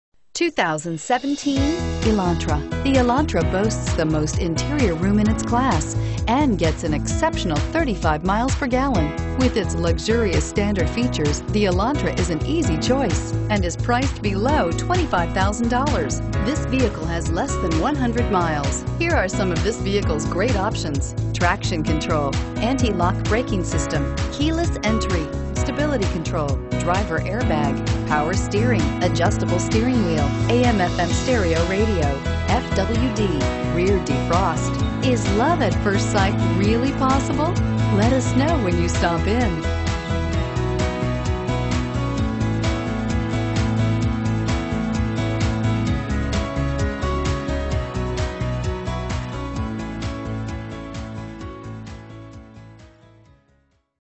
2017 Hyundai Elantra Irving TX HHH000255
2017 Hyundai Elantra

Freeman Auto
1840 E Airport Fwy

Looking for the right 4dr car? Today could be your lucky day. With 6 miles, this Phantom Black, 2017 Hyundai Elantra equipped with Automatic transmission could be yours. Request more information and set up a test drive right away.